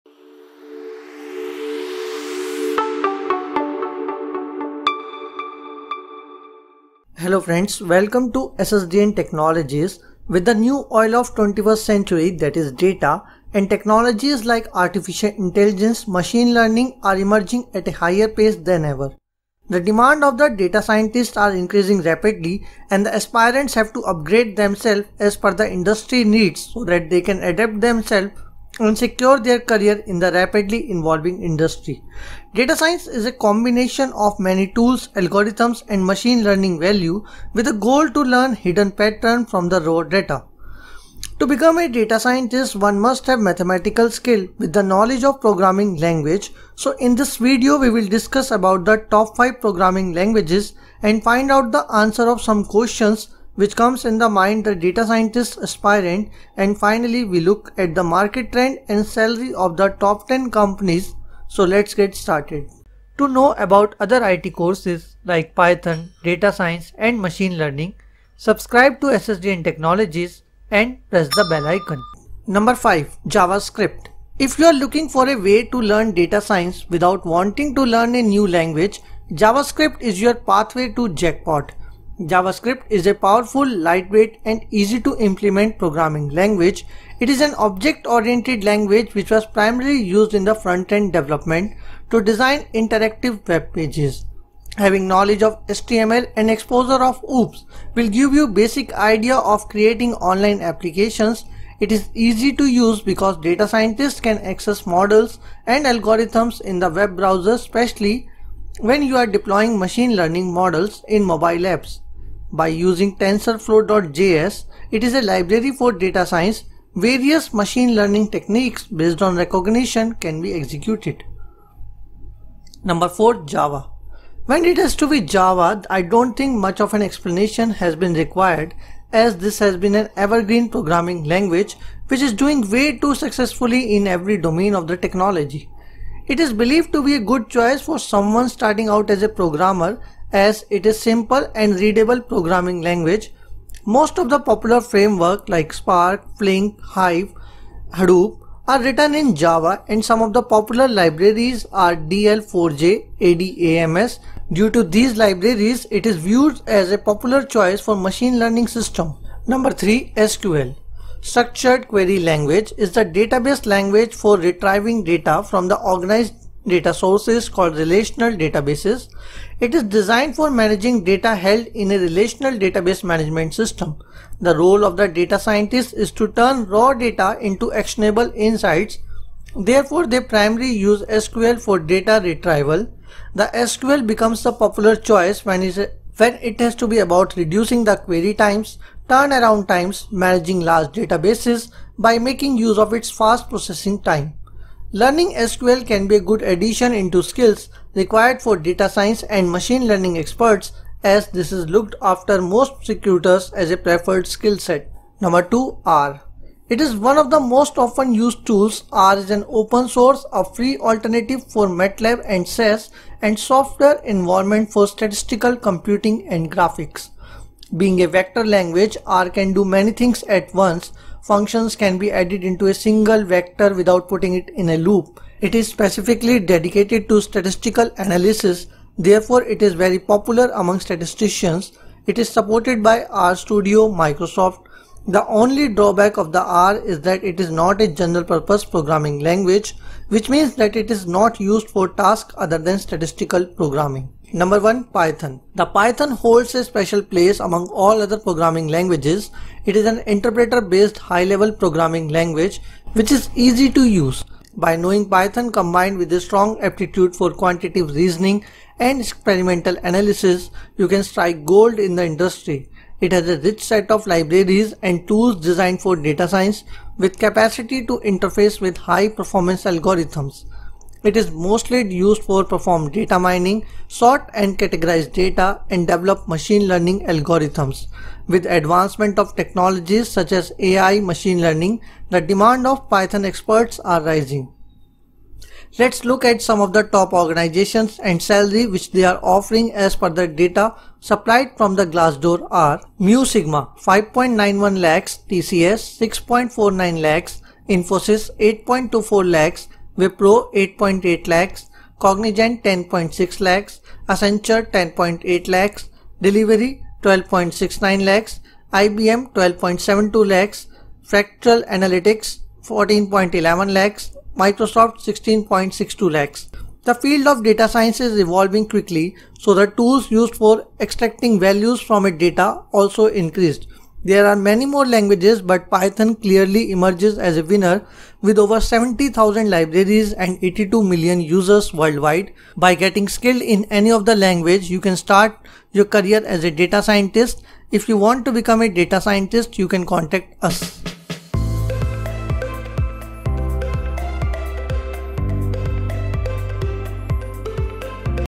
Programming Language For Data Science | Which Language is Better For Data Science
Kick Start your career in Data Science by learning programming languages. Explore which Programming language is better for data science. Watch full tutorial.